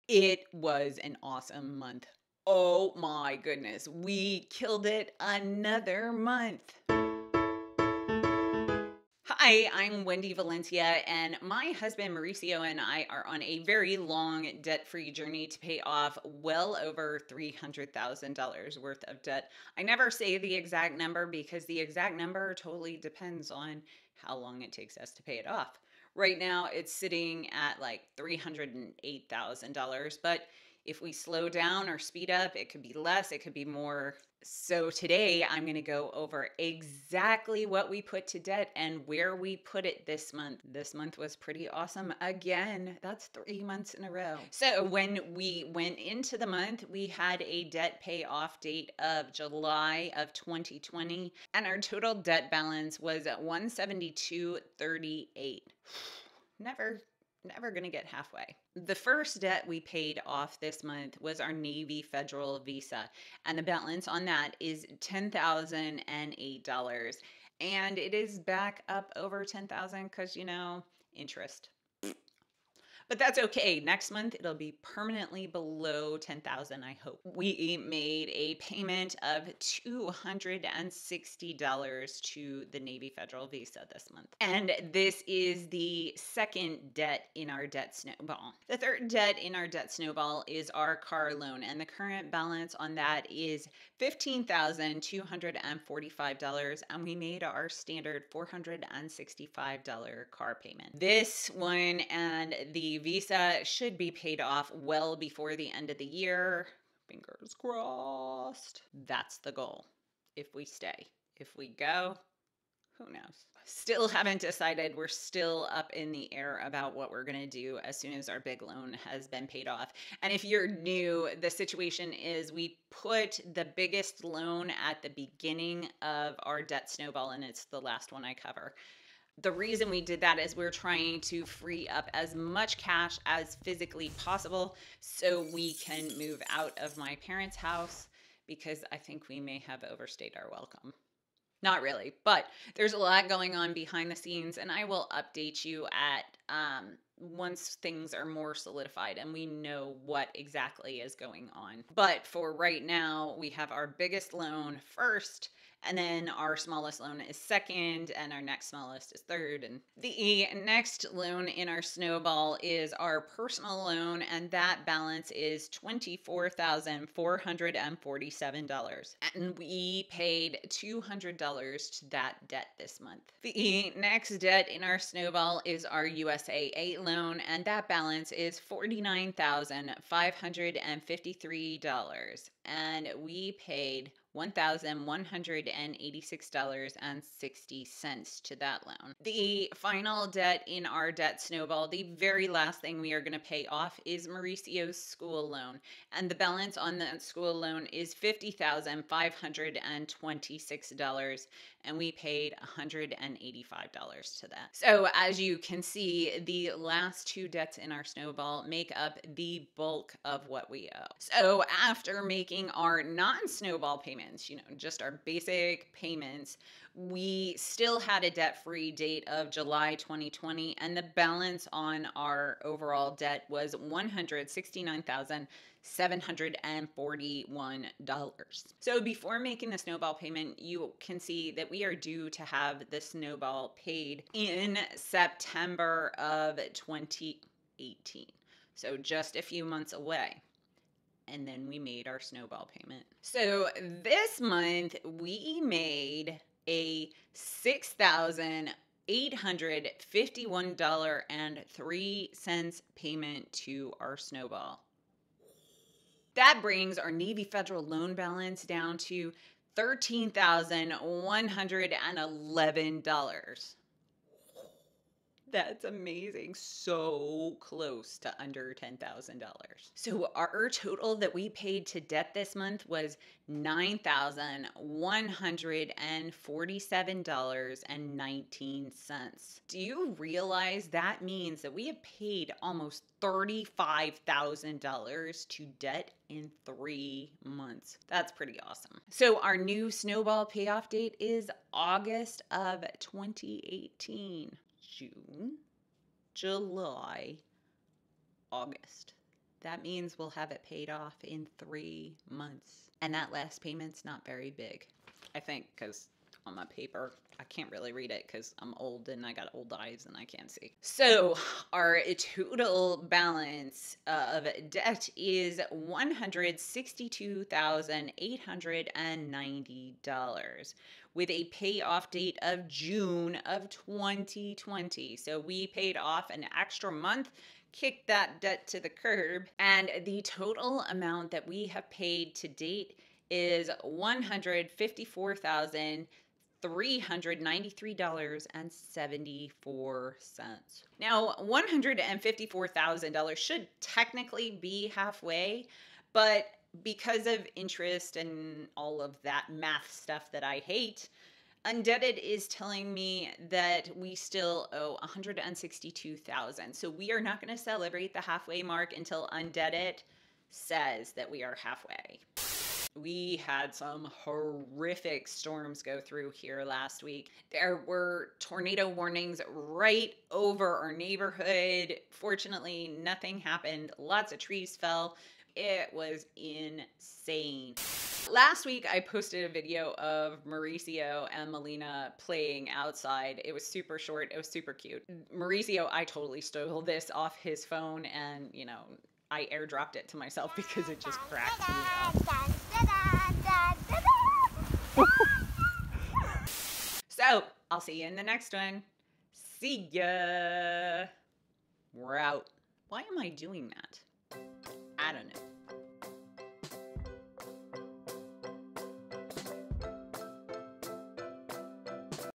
Debt Snowball Update 2018: AMAZING 3 Month Streak
In this debt snowball update (2018 of course) we talk about our amazing 3 month pay off streak!   We have put more than $35,000 to debt in the last 3 months and it feels AMAZING.  We are doing the debt snowball Dave Ramsey style and using the debt snowball app, undebt.it to get there.